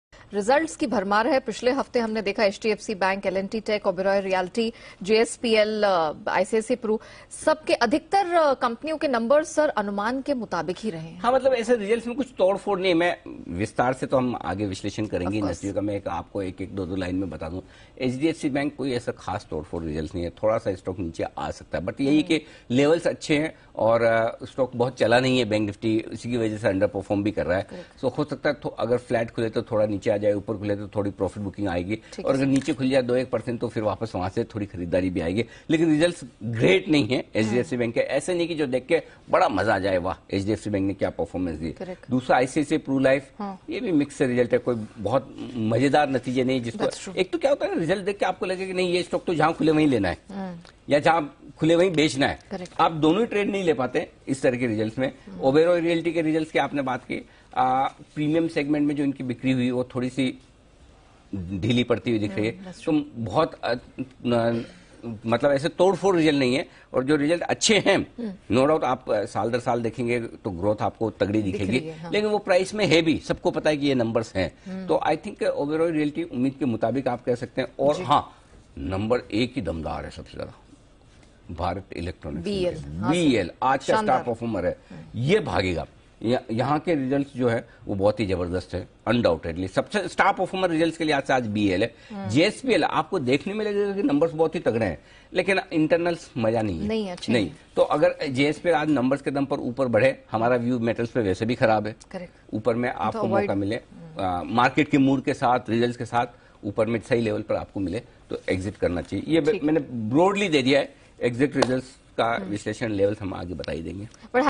What will happen to the HDFC Bank stock after the results? what to do in JSPL, Anil Singhvi Details
🌐नतीजों के बाद HDFCBank के शेयर में क्या होगा?

JSPL में क्या करें❓

✨ICICI Prudential और Oberoi Realty के नतीजे कैसे रहे❓

जानिए अनिल सिंघवी से..